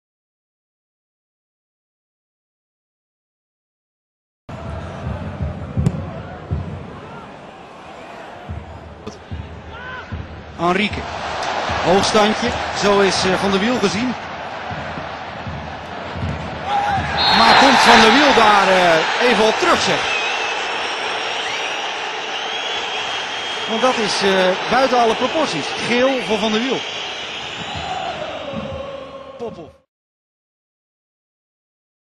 Paulo Henrique dá drible desconcertante no Campeonato Holandês
Apesar da derrota por 2 a 0 para o Ajax, o Heerenveen teve um jogador destacado: o atacante Paulo Henrique, que fez boa exibição e deu um drible em dois jogadores adversários, sendo parado apenas com falta. Veja o vídeo.
Data: 13/02/2010